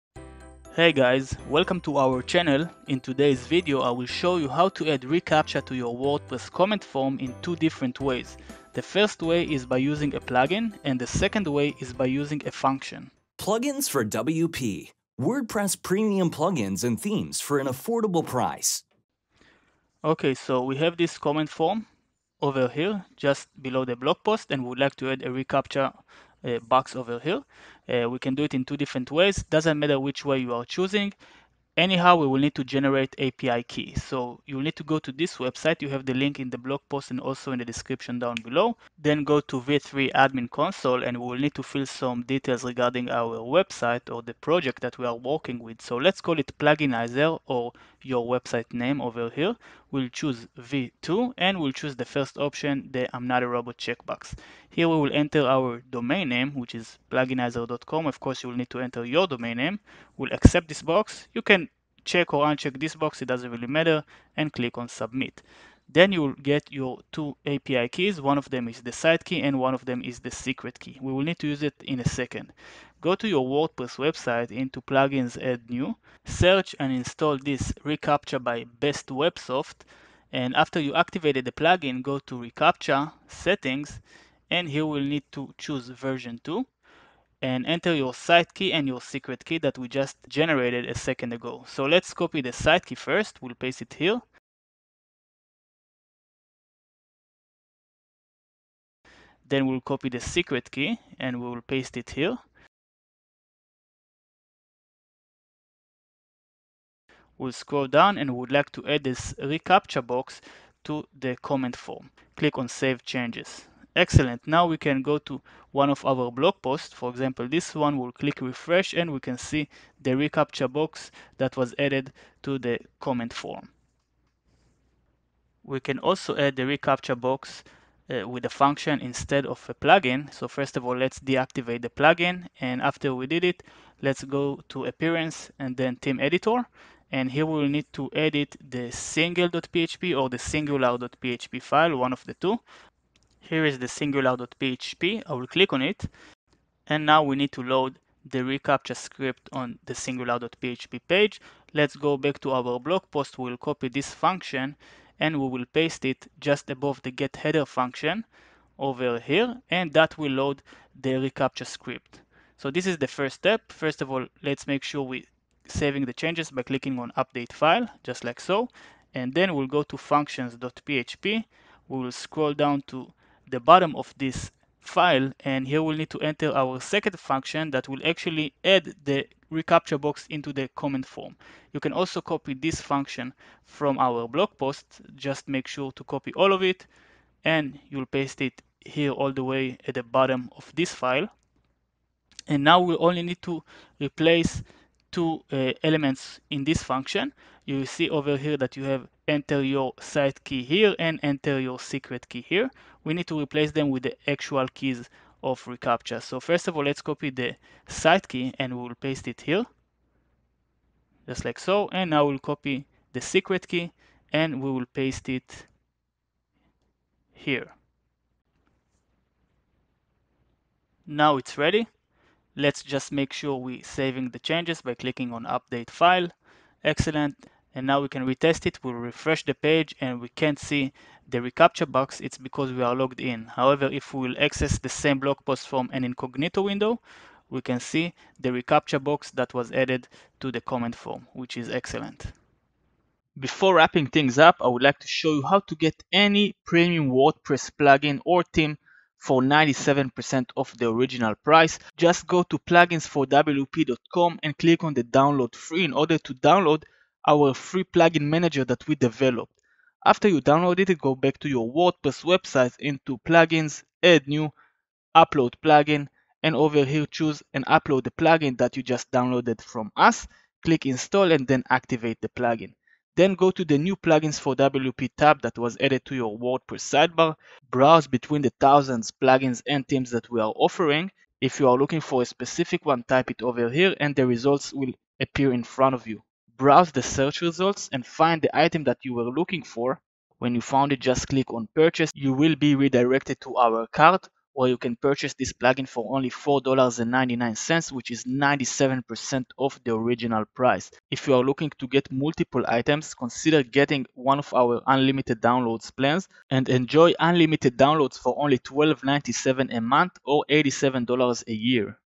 Add reCaptcha to WordPress Comment Form With or Without a Plugin (Prevent Spam Comments)
In this video, you will learn how to add a reCaptcha box to your WordPress comment form with or without a plugin.

The first way is by using a plugin and the second way is by using a function.